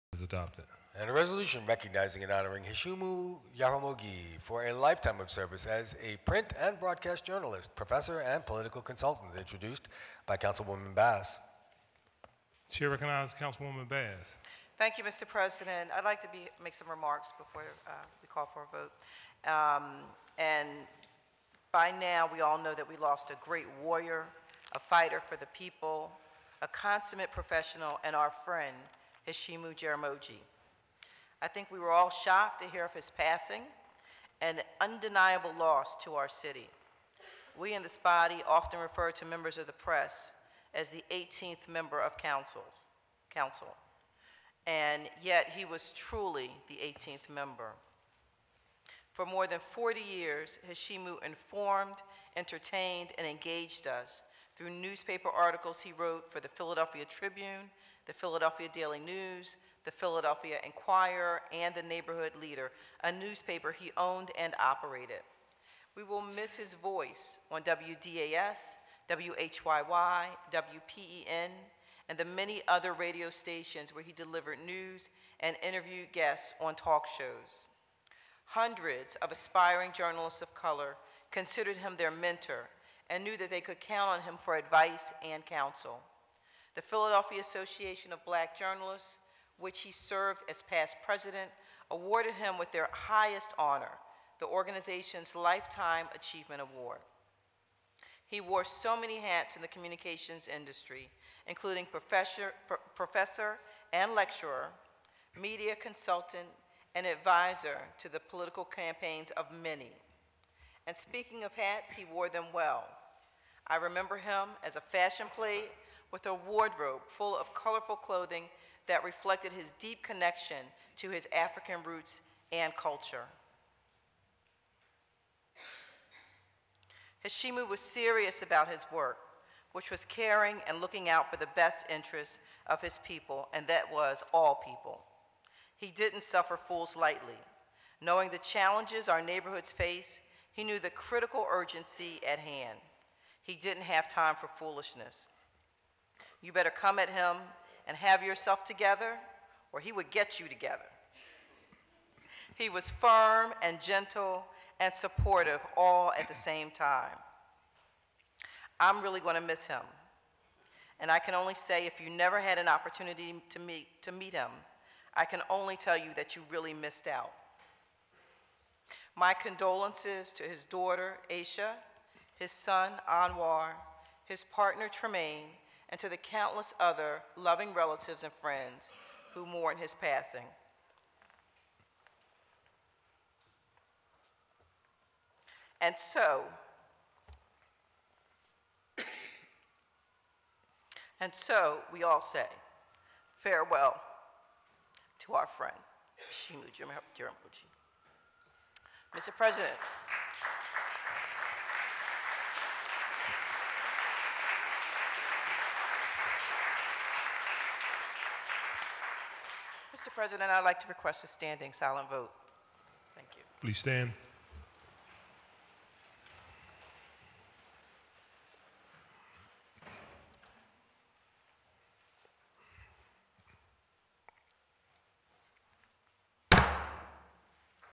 Councilmember Bass Remembers Heshimu Jaramogi 1-23-2020
From the Stated Meeting of Philadelphia City Council held January 23, 2020:

Councilmember Cindy Bass' (8th District) floor tribute to the late Philadelphia Journalist Heshimu Jaramogi.